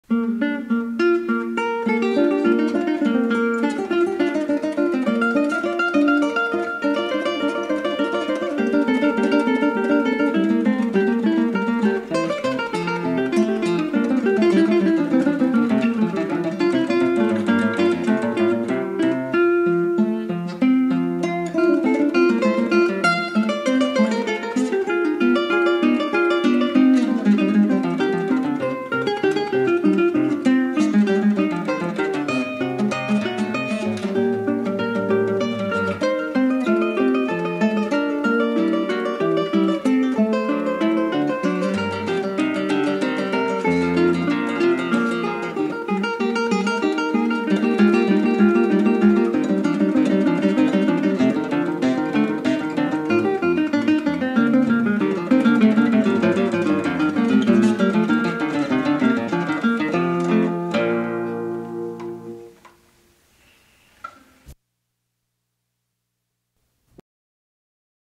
Rolando Valdes-Blain and Mark Delpriora performing a Bach Invention
Rolando was in his early 80s at the time of this recording.
We gave a few recitals in the New York area and this is one of the few recordings we made.